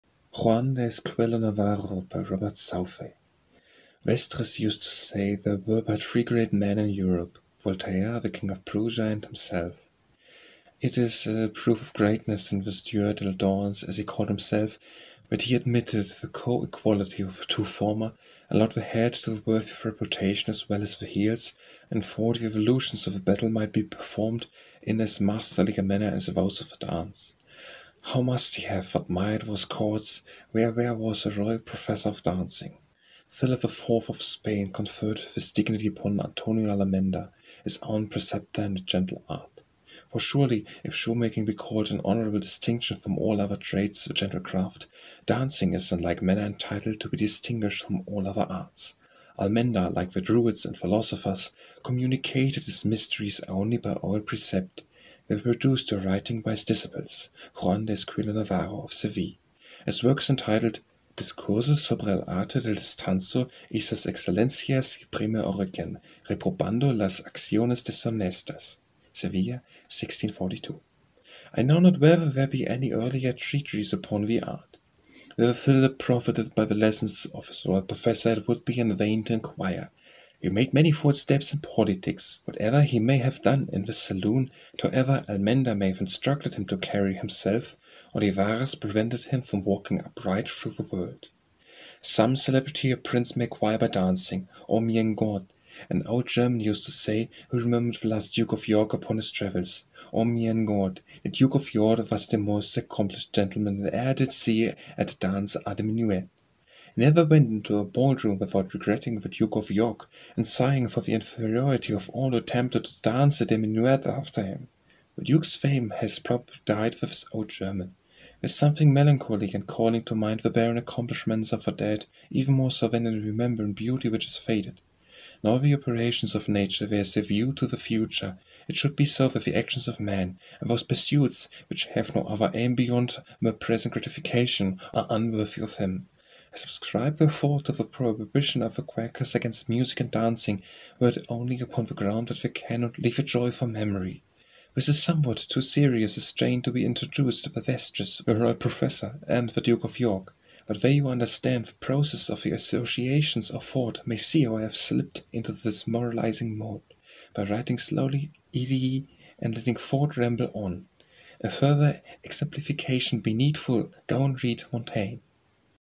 Juan de Esquivel Navarro - Robert Southey
Vestris used to say there were but three great men in Europe: Voltaire, the king of Prussia, and himself. It is a a proof of greatness in this Dieu de la Dance as he called himself, that he admitted the co-equality of the two former, allowed the head to be worthy of reputation as well as the heels, and thought the evolutions of a battle might be performed in as masterly a manner as those of a dance. How must he have admired those courts where there was a royal professor of dancing! Philip IV. of Spain conferred this dignity upon Antonio Almenda, his own preceptor in the gentle art; for surely if shoemaking be called in honourable distinction from all other trades the gentle craft, dancing is in like manner entitled to be distinguished from all other arts. Almenda, like the druids and philosophers, communicated his mysteries only by oral precept; they were reduced to writing by his disciple Juan de Esquivel Navarro, of Seville. His work is entitled, Discurso sobre el arte del Danzado y sus excellencias y primer origen, reprobando las acciones deshonestas. Sevilla, 1642. I know not whether there be any earlier treatise upon the art.

Whether Philip profited by the lessons of his royal professor it would be in vain to inquire. He made many false steps in politics, whatever he may have done in the saloon; and however Almenda may have instructed him to carry himself, Olivares prevented him from walking uprightly through the world.

Some celebrity a prince may acquire by dancing. Oh mine Gott! an old German used to say, who remembered the last Duke of York upon his travels, Oh mine Gott! de Duke of York vas de mose accompleesh gentleman dat ever I did see at dance-a de minnuett! He never went into a ball room withing regretting the Duke of York, and sighing for the inferiority of all who attempted to dance a de minnuett after him. The Duke's fame has probably died with this old German. There is something melancholy in calling to mind the barren accomplishments of the dead, even more so than in remembering beauty which is faded. In all the operations of nature there is a view to the future; it should be so with the actions of man, and those pursuits which have no other aim beyond mere present gratification, are unworthy of him. This is somewhat too serious a strain to be introduced by Vestris, the royal professor, and the Duke of York; but they who understand the process of the associations of thought may see how I have slipt into this moralizing mood, by writing slowly, idiy, and letting thought ramble on. If further exemplification be needful, go and read Montaigne.